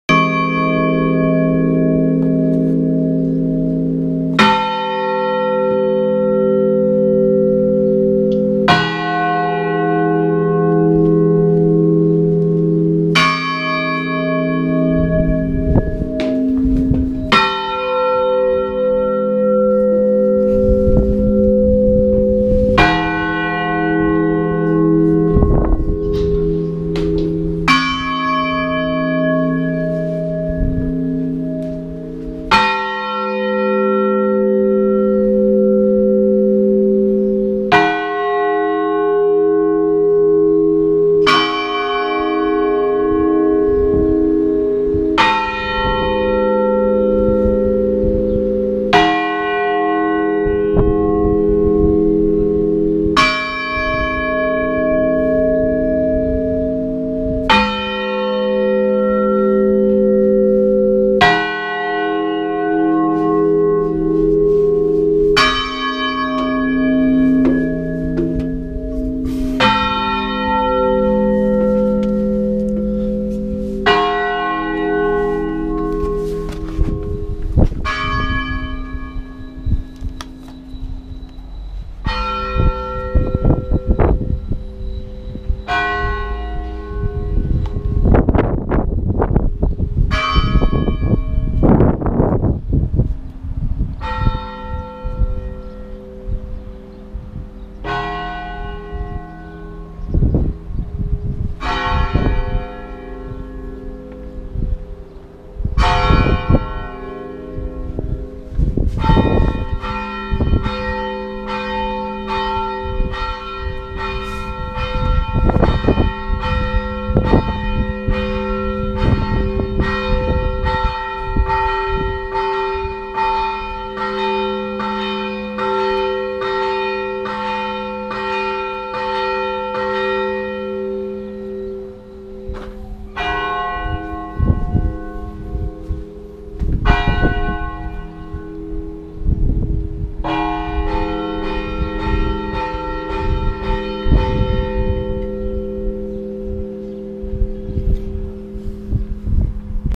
(TERRAZA) campanas de Puzol // Tercer toque de muerto a mujer y entrada
Toque de muerto de mujer desde la terraza